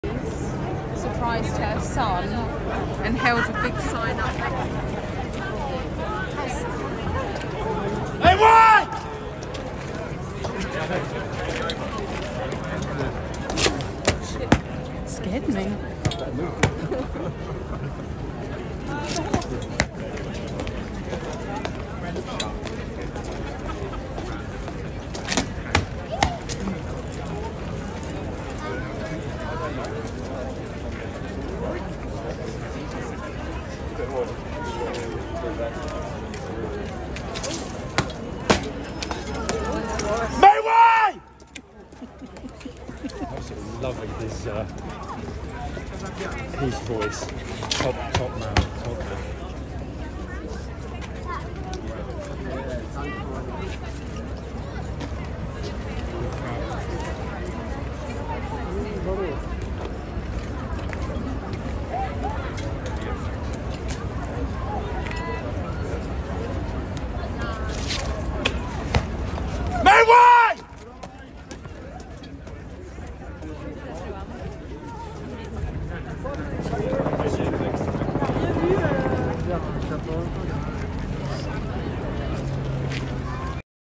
"The King’s Guard Shouts, Shouts and Shouts Crazy Loud at Tourist. ( make Way )😵😡
The Blues and Royal King’s Guard Shout loud three Times at Tourist,